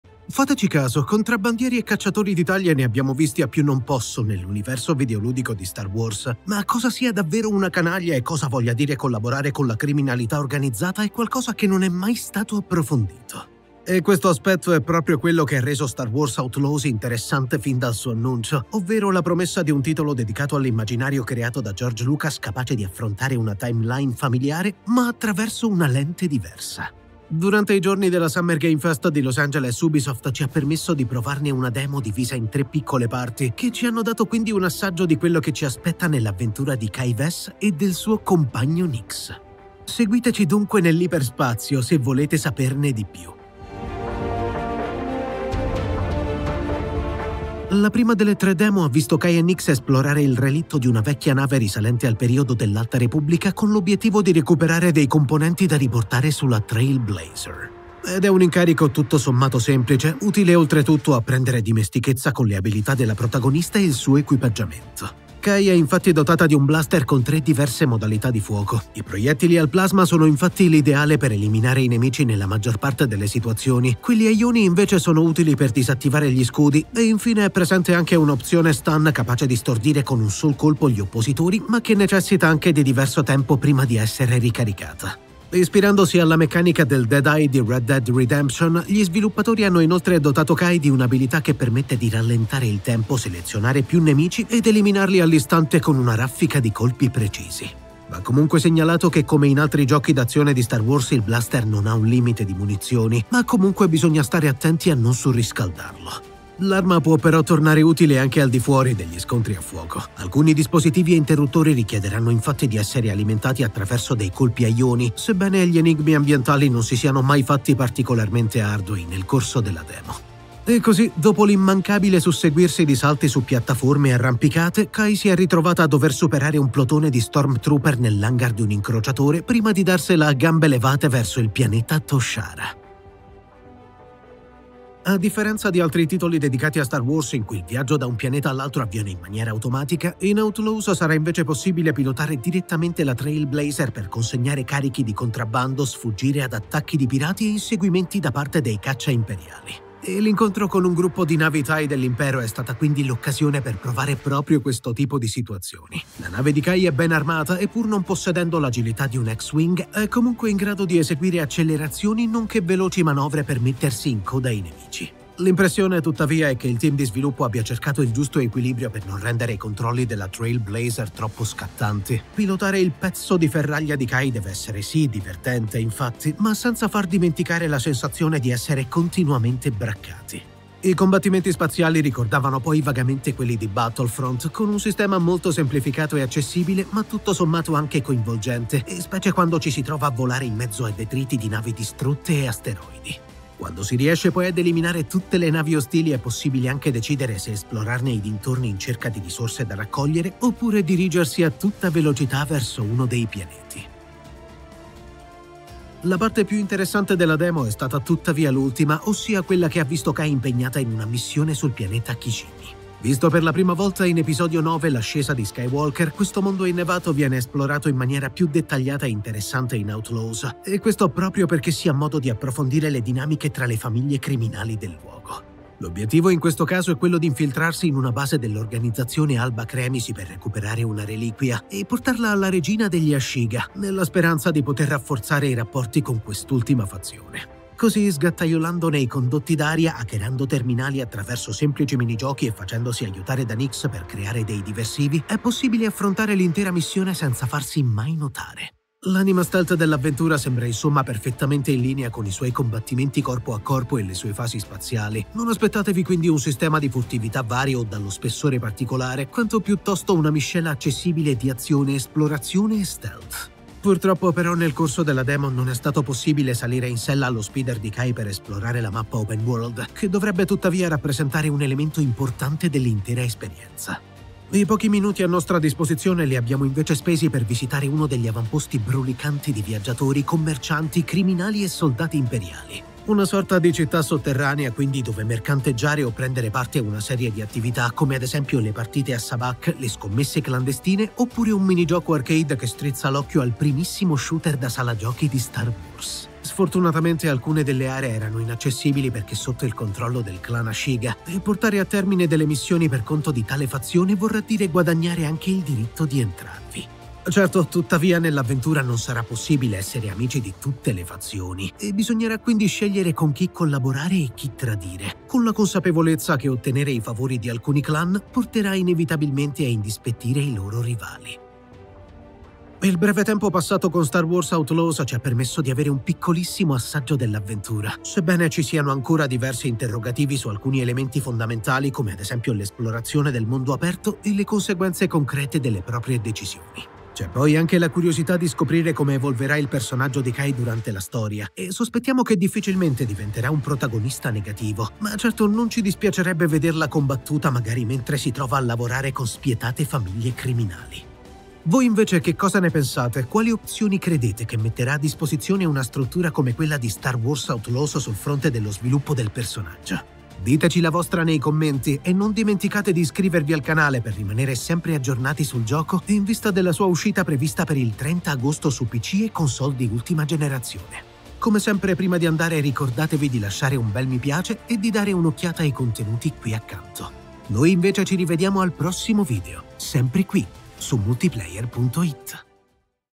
Star Wars Outlaws, provato l'open world Ubisoft
Mancano una manciata di mesi all'uscita di Star Wars Outlaws e durante la cornice della Summer Game Fest ne abbiamo provato un piccolo assaggio.
Vi raccontiamo le nostre impressioni dopo aver giocato alla prima demo dell'open world di Ubisoft.

00:00 - Star Wars Outlaws: PROVATO a Los Angeles
00:44 - Infiltrazione
02:14 - Nello spazio
03:25 - A tu per tu coi clan
05:22 - Certezze e dubbi